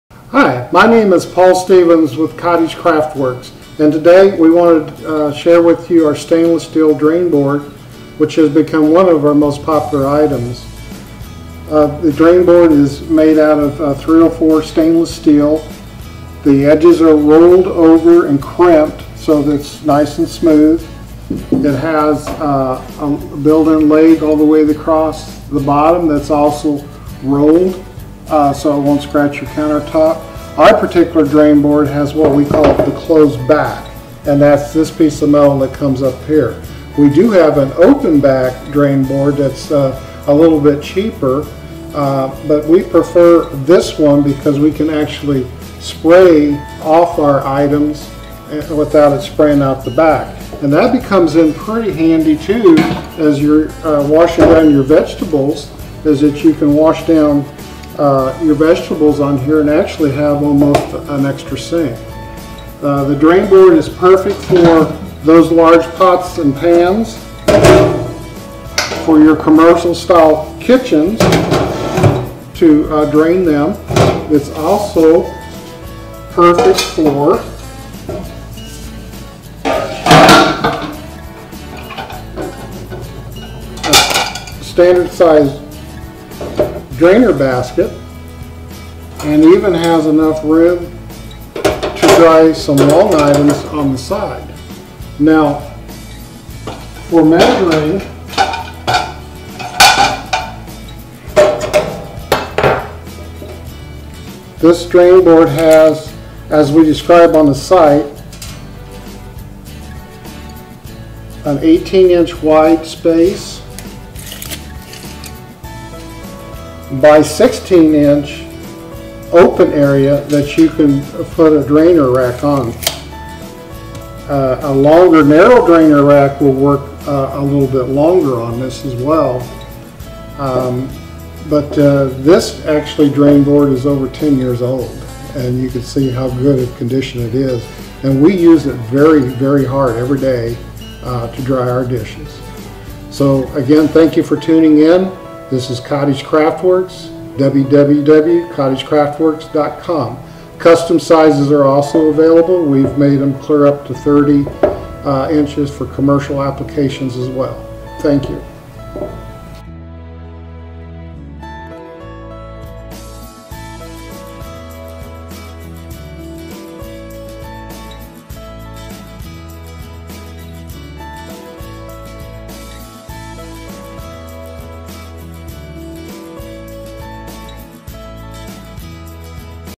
STAINLESS STEEL KITCHEN SINK OPEN BACK DRAINBOARD USA MADE
This is just like our standard drainboard except the back edge is left open. This saves on labor thus we are able to provide this option at a lower price.

The open back design also comes in handy if you need to dry long handle items where the handles would be harmed if they become wet.

Our Amish made Stainless Steel drainboard sits next to your sink allowing all the water to quickly drain back into the sink. The drain board is universal to fit either the right or left sides of a kitchen sink.